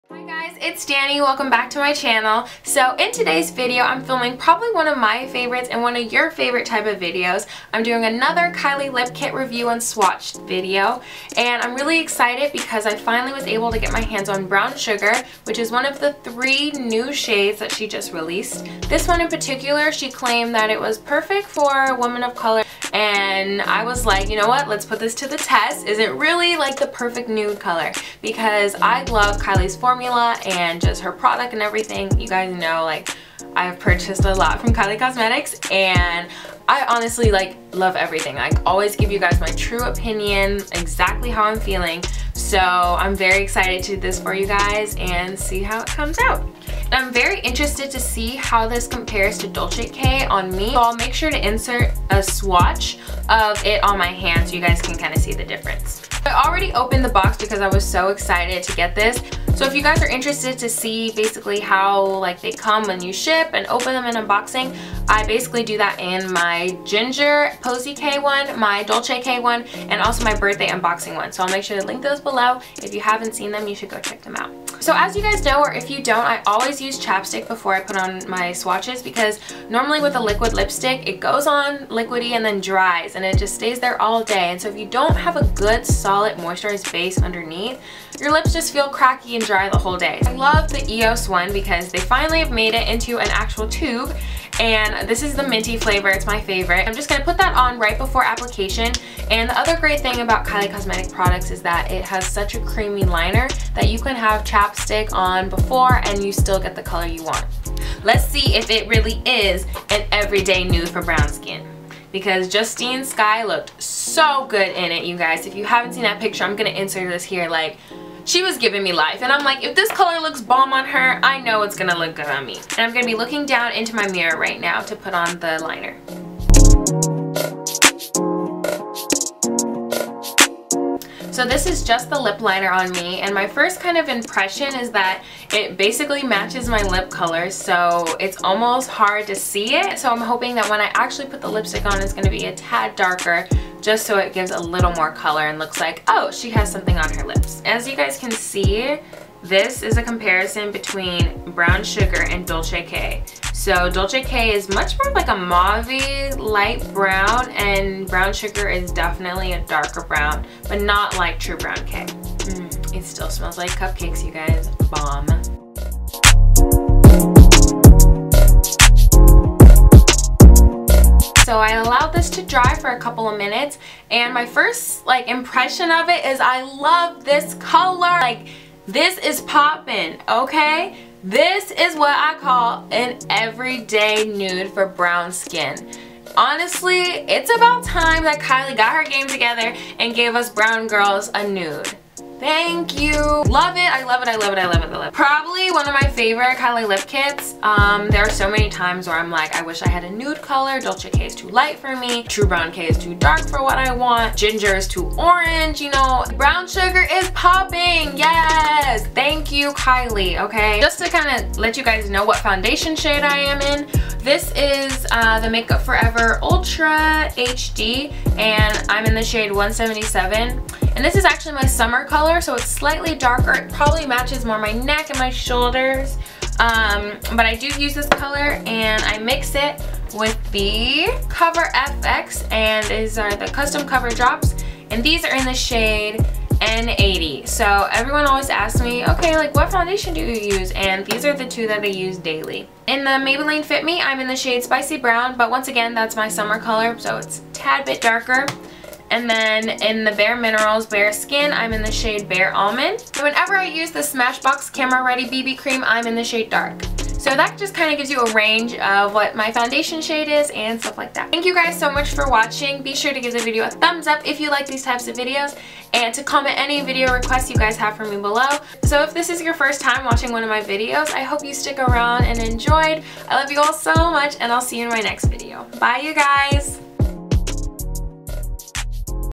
BROWN SUGAR KYLIE LIP KIT ♡| Perfect Nude for Brown Skin?
HI LOVES!!
Honestly can't tell you how excited I am for this video! Let me know what you guys think of it!

FOLLOW ME SO WE CAN CHAT! :)
LOVE ALL OF YOU!!!
xo,
Dani